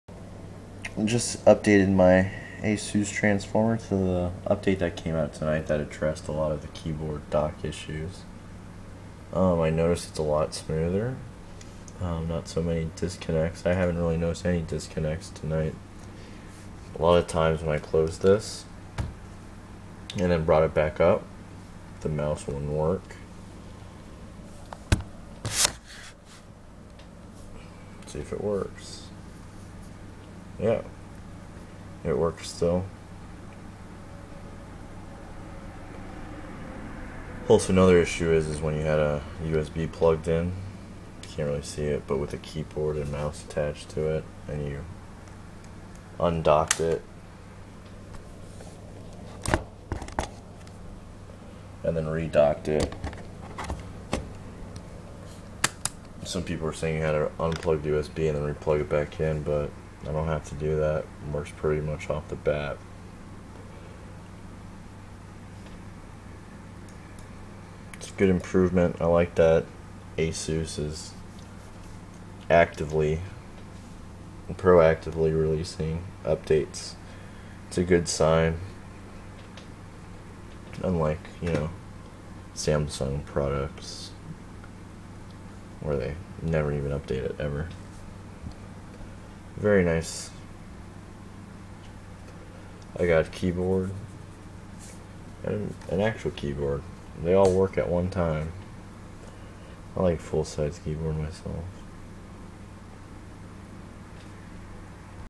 Asus EEE Pad Transformer 8.2.3.12 keyboard dock update
Asus released the 8.2.3.12 firmware update for the Asus EEE Pad Transformer which addresses mainly the keyboard dock bugs. Althought the firmware update doesnt include the keyboard dock battery meter that was in the beta, its a good sign that asus is actively releasing updates for bugs.